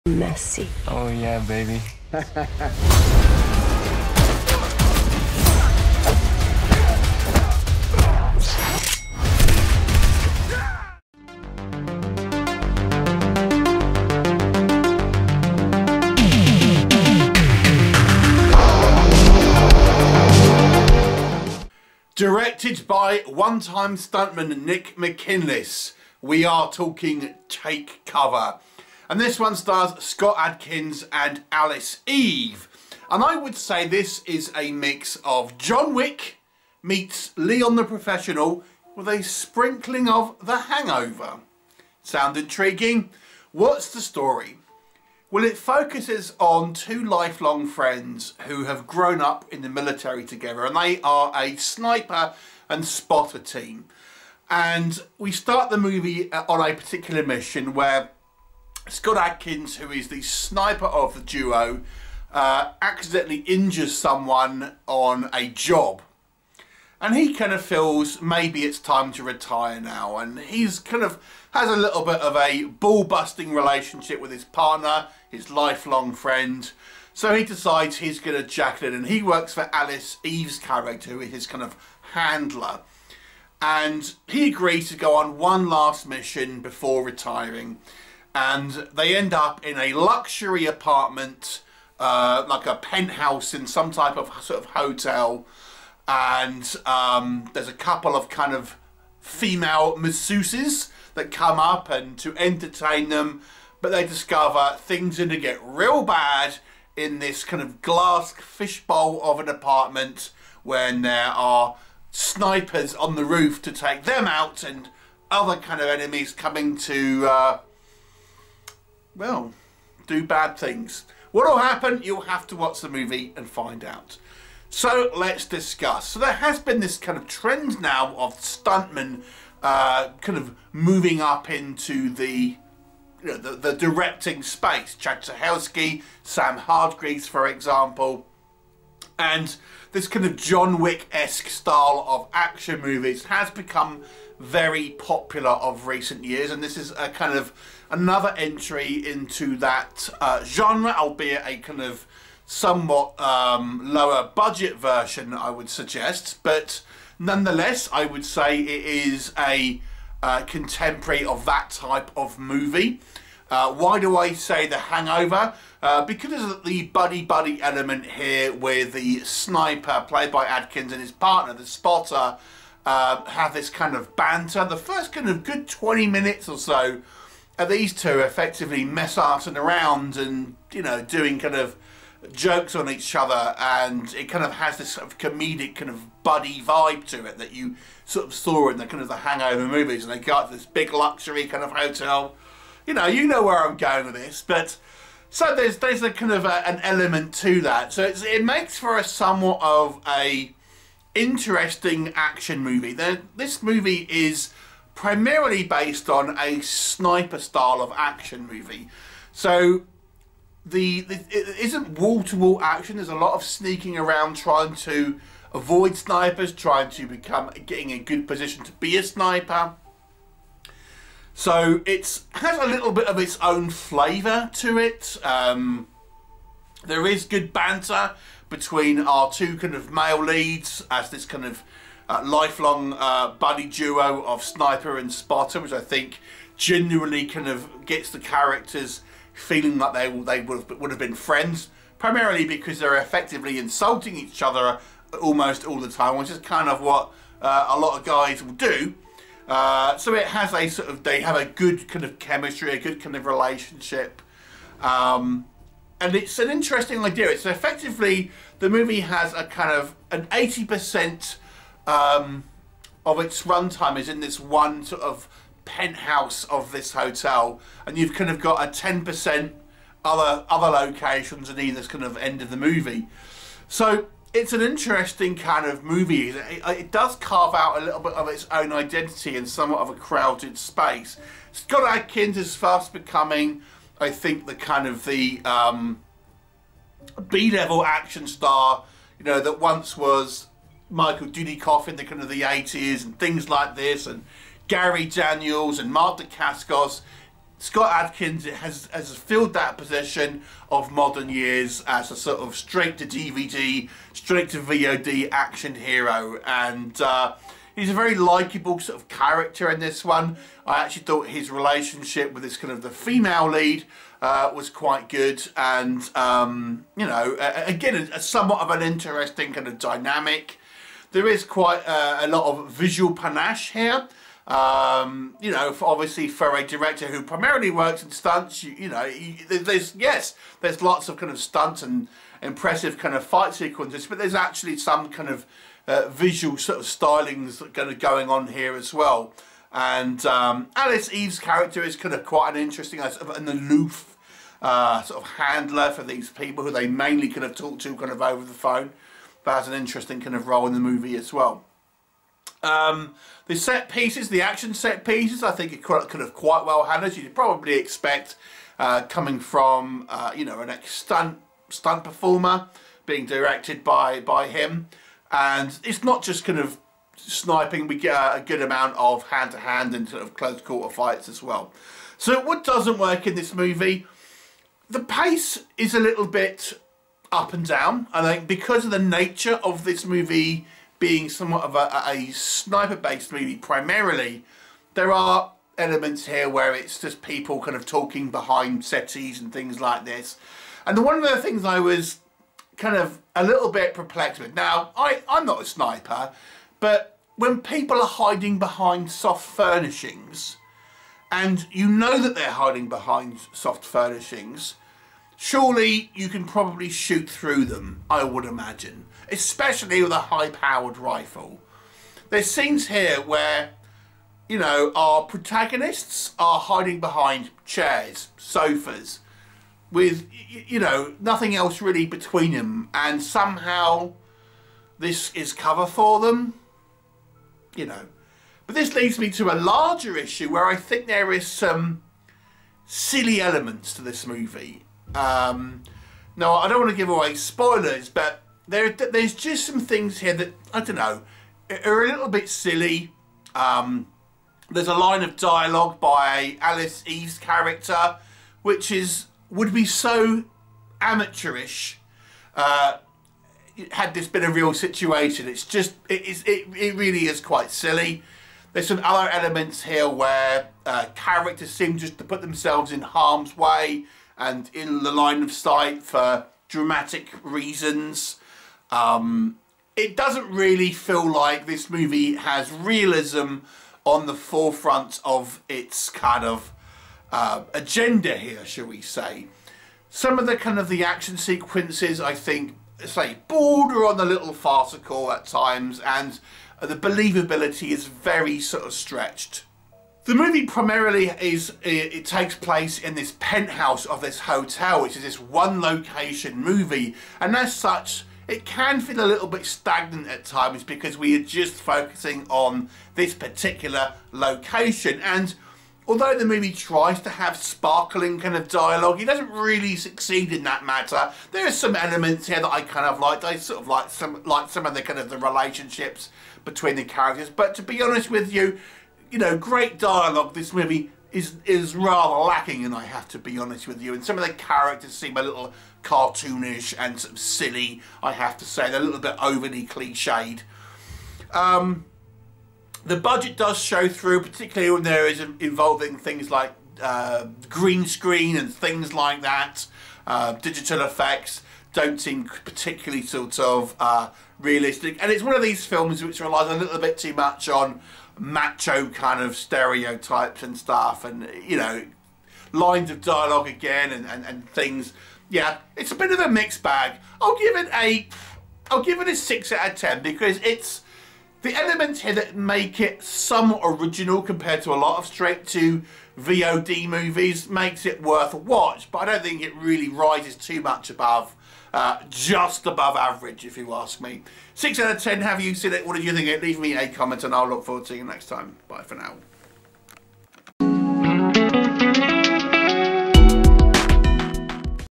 TAKE COVER ( 2024 Scott Adkins ) John Wick Inspired Sniper Action Movie Review
Synopsis: Scott Adkins has got no cover, no back-up, and no way out in this claustrophobic action thriller. Sam Lorde has spent a lifetime killing people. After a hit gone wrong, he finds himself disillusioned and lacking purpose, deciding to retire as a hitman. His final job however, results in him being a target himself. With little to no cover, he must fight with everything he has to survive. Starring Scott Adkins (One More Shot), Jack Parr (Peaky Blinders), Alice Eve (Bombshell), Madalina Bellariu Ion (The Young Pope) and Billy Clements (Meg 2: The Trench). Take Cover is written by Joshua Todd James (Pound of Flesh) and directed by Nick McKinless (The Iron Grip).
Take Cover is available on Digital Platforms and DVD 14 October. Distributed by Signature Entertainment